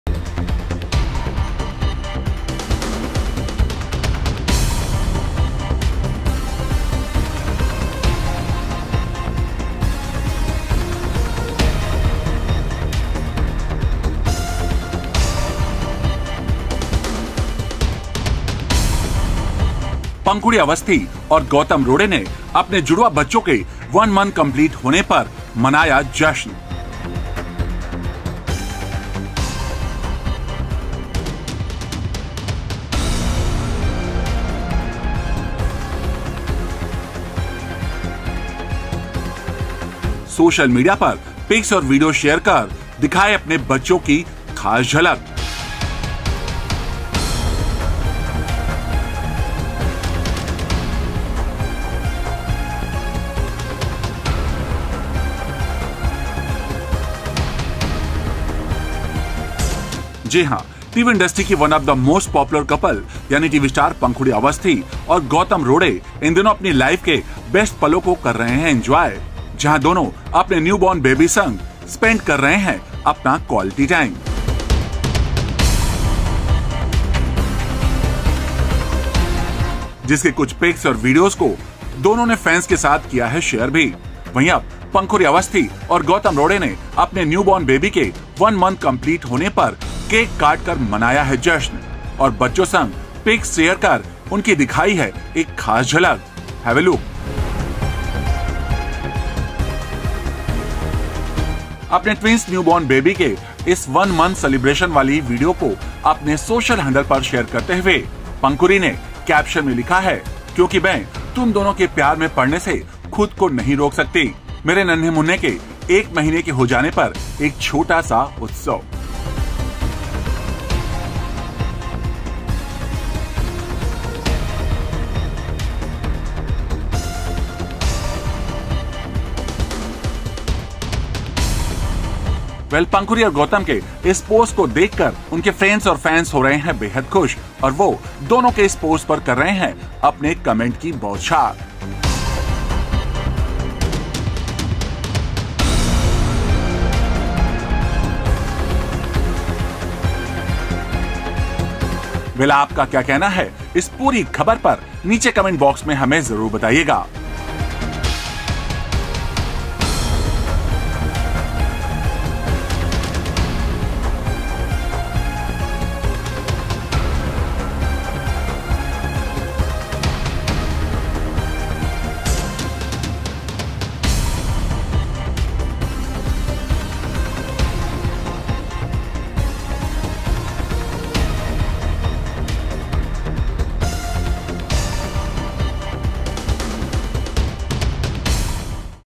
Pankhuri Awasthy and Gautam Rode celebrate one month of their twin babies | TV News
Television News- Panhuri Awasthy and Gautam Rode welcome twins and the couple has celebrated one month birthday of their twin babies. Pankhuri took to her social media and shared a cute video of the same. Listen in!